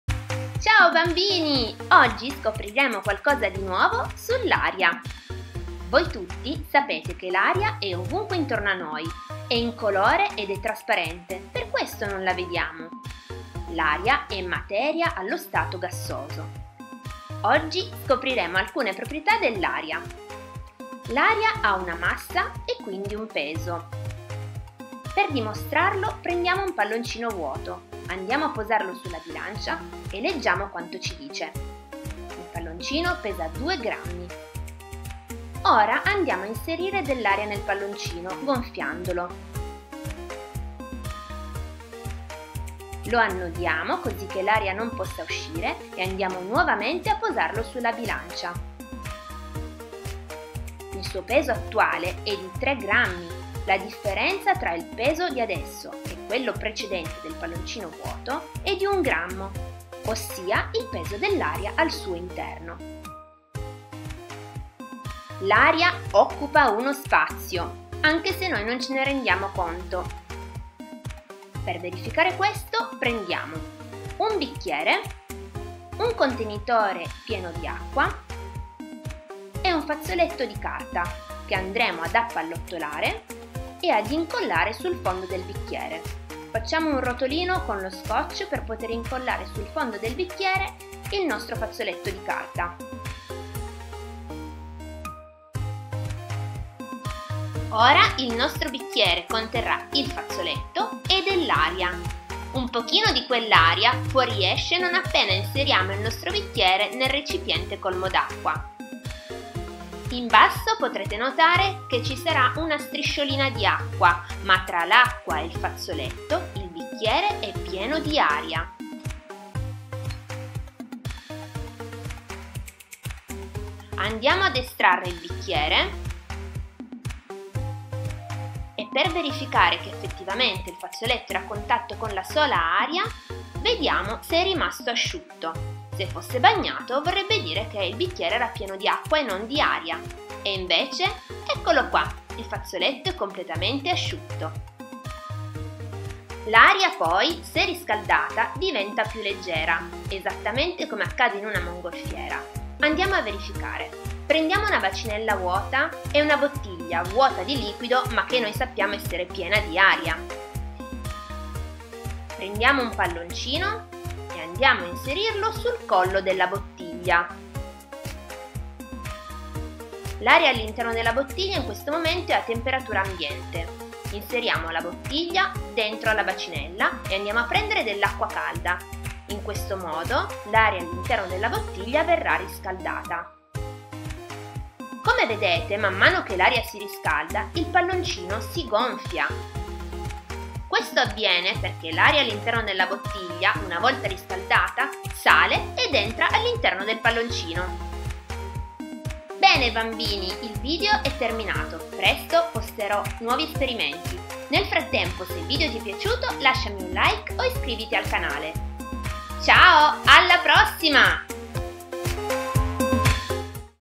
Le proprietà dell'aria. Classe 3^ ESPERIMENTI E SPIEGAZIONI
Ma l’Aria Esiste Davvero? Scopri i suoi Segreti Incredibili!
L’aria non si vede… ma è ovunque!
🌬️ In questo video divertente e facile da seguire, scopriamo insieme che cos’è l’aria, dove si trova e quali sono le sue proprietà principali. 💨
Perfetto per i bambini di terza elementare, questo viaggio nel mondo invisibile dell’aria è pieno di curiosità, esperimenti semplici e spiegazioni chiare.
👧👦 Adatto per la scuola o da guardare a casa con mamma e papà!

Contenuti del video:
✅ Cos’è l’aria
✅ Dove si trova
✅ Le proprietà dell’aria
✅ Esperimenti facili e divertenti

LINK UTILI:
00:00 introduzione
00:06 cos'è l'aria
00:21 l'aria ha una massa e un peso, esperimento
01:11 l'aria occupa uno spazio, esperimento
02:25 l'aria calda è più leggera, esperimento
03:24 call to action

Esperimenti per verificare le proprietà dell'aria. Classe 3^ primaria, SCIENZE.
Maestra Marina83.
scientific method experiments
science expertiments
interesting science experiments